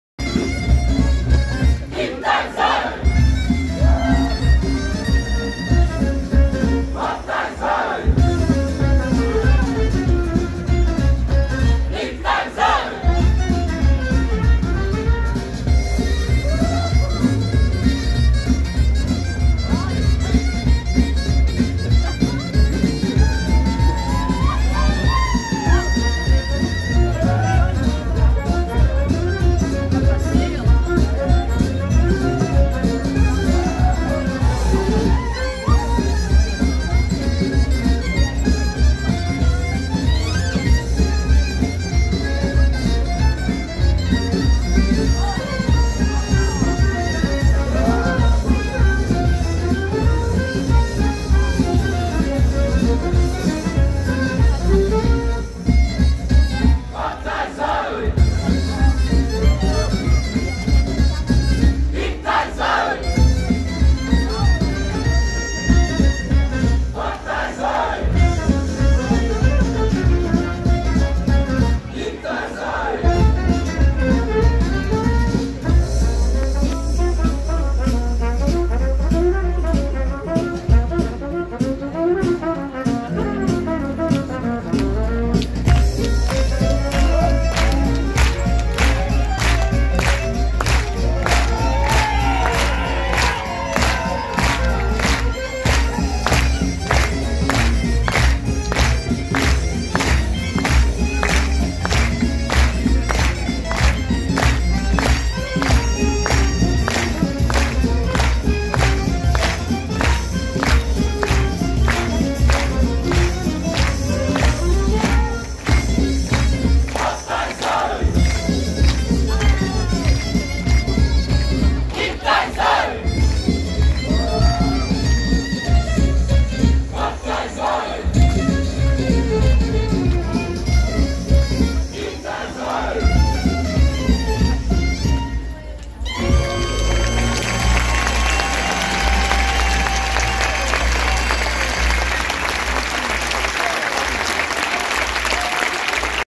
Cercle de St Malo / La Tribu JIPSIAN / Festival Interceltique de Lorient 2023
Le cercle celtique Quic en Groigne Saint-Malo avec son spectacle de rue "La tribu JIPSIAN"  au Festival Interceltique de Lorient 2023 🤍🖤

Vidéo Norbert Fest Noz / Crédit audio Kendirvi